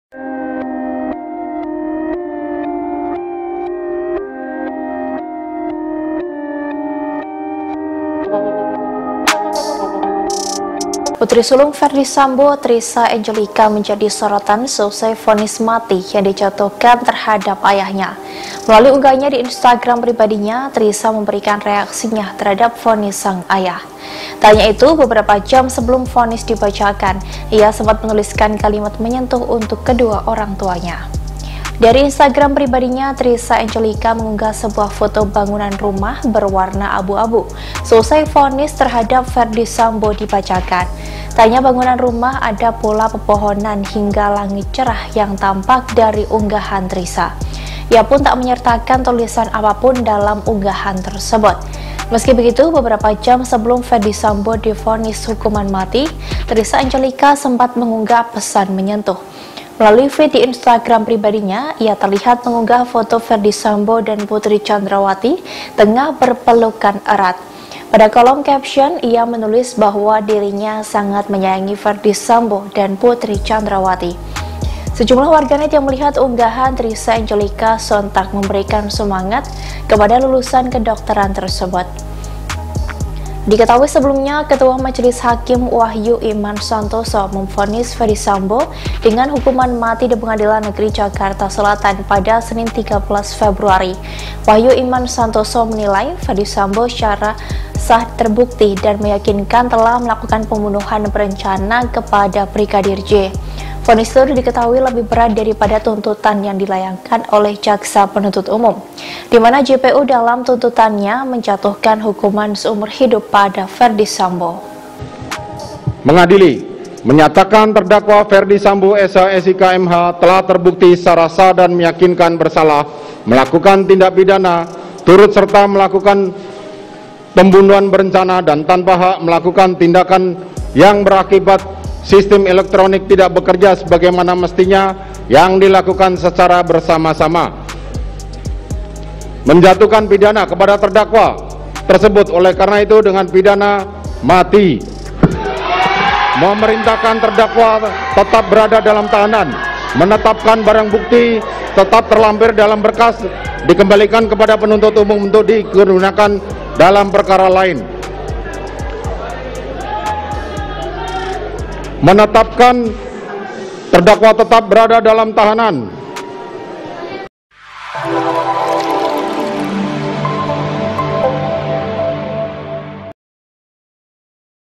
Pesan Menyentuh Putri Sulung Ferdy Sambo Usai Sang Ayah Divonis Mati dan Unggah Foto Ini
Putri sulung Ferdy Sambo, Trisha Eungelica menjadi sorotan seusai vonis mati yang dijatuhkan terhadap ayahnya.

Melalui unggahannya di Instagram pribadinya, Trisha memberikan reaksinya terhadap vonis sang ayah.

Tak hanya itu, beberapa jam sebelum vonis dibacakan, ia sempat menuliskan kalimat menyentuh untuk kedua orangtuanya.